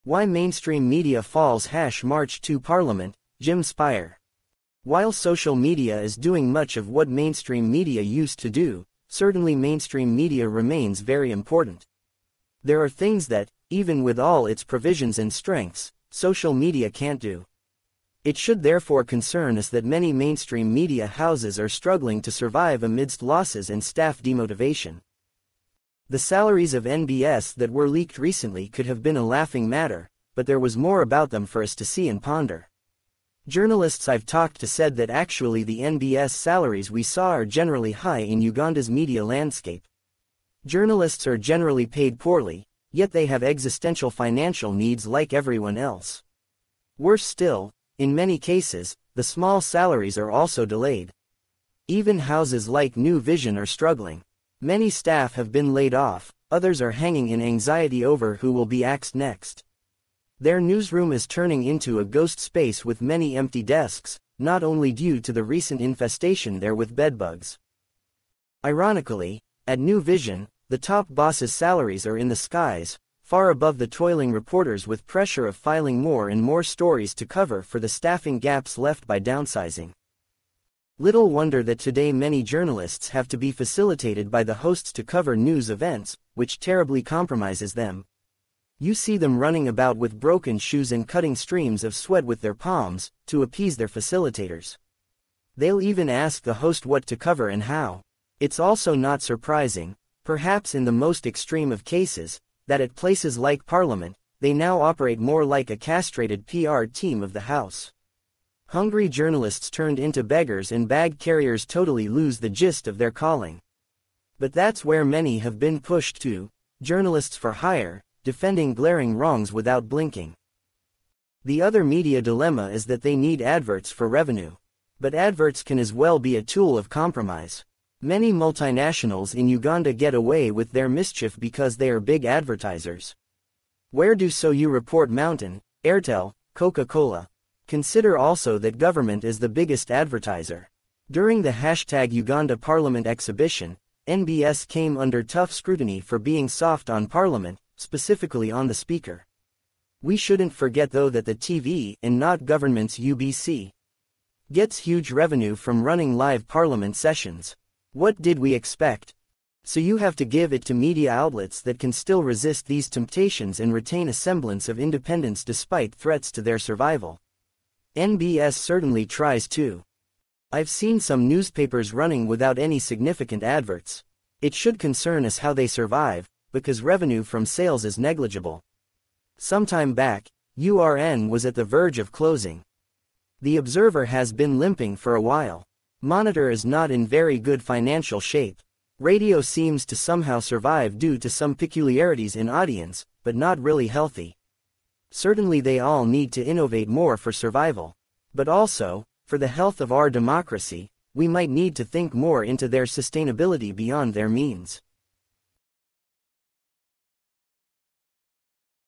Why mainstream media falls march2parliament - Jim Spire
While social media is doing much of what mainstream media used to do, certainly mainstream media remains very important. There are things that, even with all its provisions and strengths, social media can’t do. It should therefore concern us that many mainstream media houses are struggling to survive amidst losses and staff demotivation.

The salaries of NBS that were leaked recently could have been a laughing matter, but there was more about them for us to see and ponder. Journalists I’ve talked to said that actually the NBS salaries we saw are generally high in Uganda’s media landscape. Journalists are generally paid poorly, yet they have existential financial needs like everyone else. Worse still, in many cases, the small salaries are also delayed. Even houses like New Vision are struggling. Many staff have been laid off, others are hanging in anxiety over who will be axed next. Their newsroom is turning into a ghost space with many empty desks, not only due to the recent infestation there with bedbugs.

Ironically, at New Vision, the top bosses’ salaries are in the skies, far above the toiling reporters with pressure of filing more and more stories to cover for the staffing gaps left by downsizing.

Little wonder that today many journalists have to be facilitated by the hosts to cover news events - which terribly compromises them. You see them running about with broken shoes and cutting streams of sweat with their palms, to appease their facilitators. They’ll even ask the host what to cover and how! It’s also not surprising, perhaps in the most extreme of cases, that at places like Parliament, they now operate more like a castrated PR team of the house. Hungry Journalists turned into beggars and bag carriers totally lose the gist of their calling. But that’s where many have been pushed to - journalists for hire, defending glaring wrongs without blinking.

The other media dilemma is that they need adverts for revenue. But adverts can as well be a tool of compromise. Many multinationals in Uganda get away with their mischief because they are big advertisers. Where do so you report MTN, Airtel, Coca-Cola, ..? Consider also that government is the biggest advertiser. During the UgandaParliamentExhibition, NBS came under tough scrutiny for being soft on Parliament, specifically on the Speaker. We shouldn’t forget though that the TV (and not government’s UBC?) gets huge revenue from running live Parliament sessions. What did we expect?

So you have to give it to media outlets that can still resist these temptations and retain a semblance of independence despite threats to their survival. NBS certainly tries too.

I’ve seen some newspapers running without any significant adverts. It should concern us how they survive, because revenue from sales is negligible. Sometime back, URN was at the verge of closing. The Observer has been limping for a while. Monitor is not in very good financial shape. Radio seems to somehow survive due to some peculiarities in audience, but not really healthy. Certainly they all need to innovate more for survival. But also, for the health of our democracy, we might need to think more into their sustainability beyond their means.